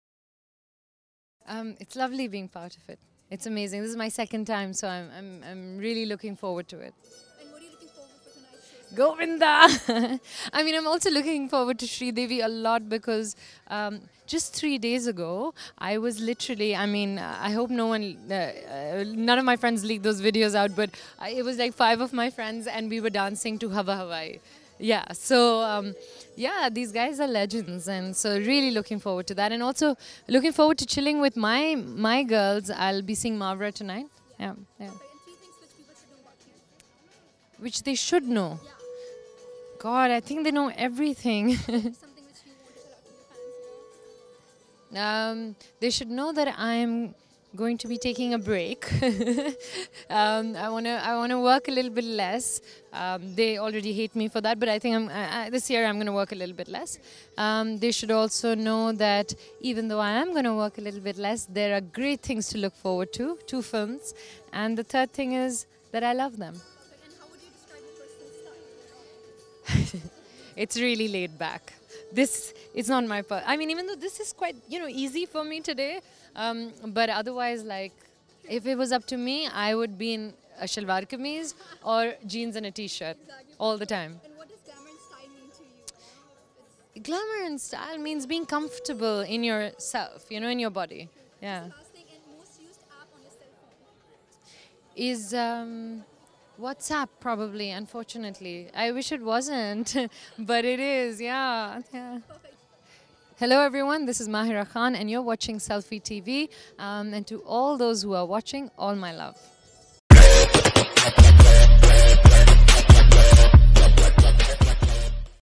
We bet you didn't know 3 things about Mahira khan | Short Interview with Mahira Khan
Short Interview with Mahira Khan and ask her about her secrets.
Enjoy the short video of Mahira Khan